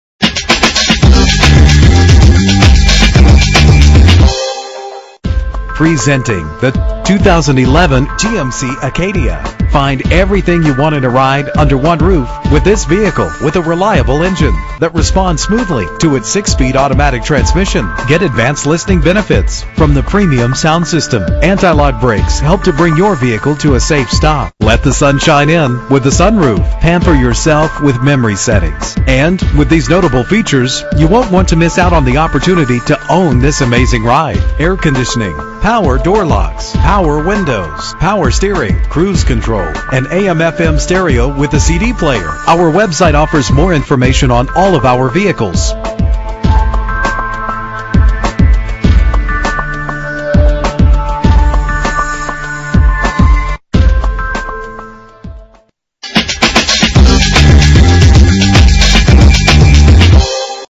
2011 GMC Acadia - Mesa AZ
Year: 2011
Make: GMC
Model: Acadia
Trim: Denali 4D SUV Sport Utility
Engine: 3.6 liter V-6
Transmission: 6-Speed Automatic
Color: White
Mileage: 5
Address:
6315 East Auto Park Drive